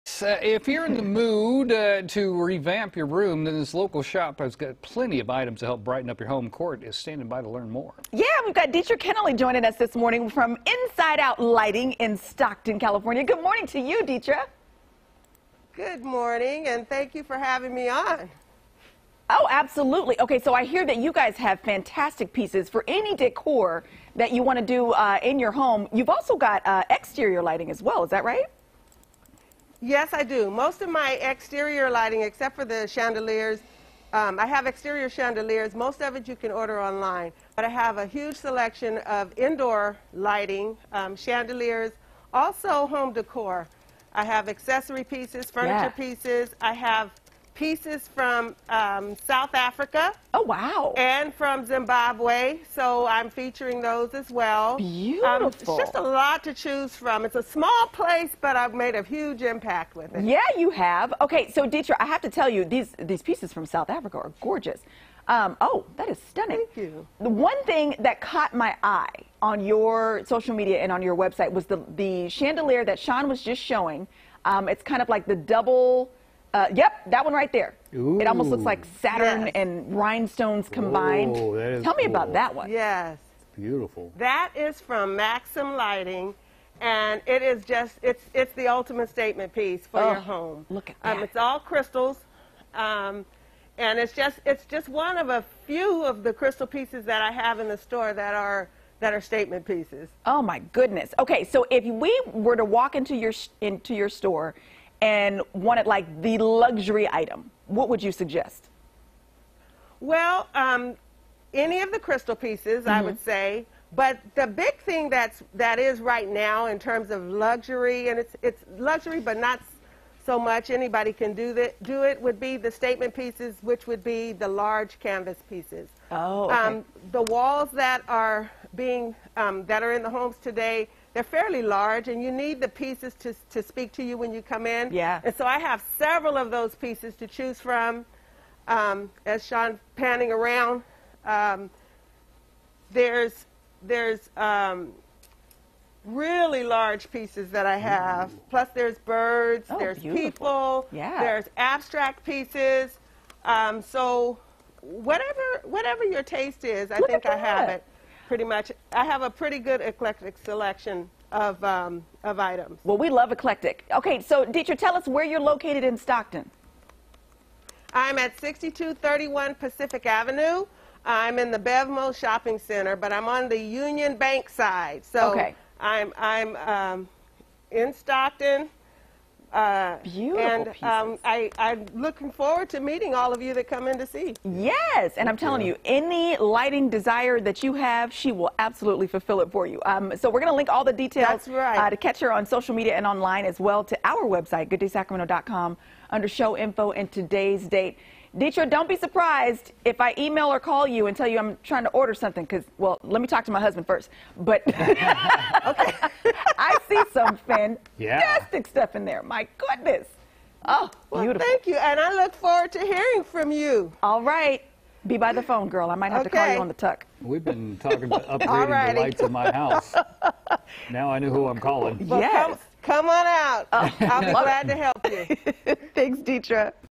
Inside Out Lighting and Decor
We're in Stockton at Inside Out Lighting and Decor and they show us how their lights can spruce up your house, inside and out!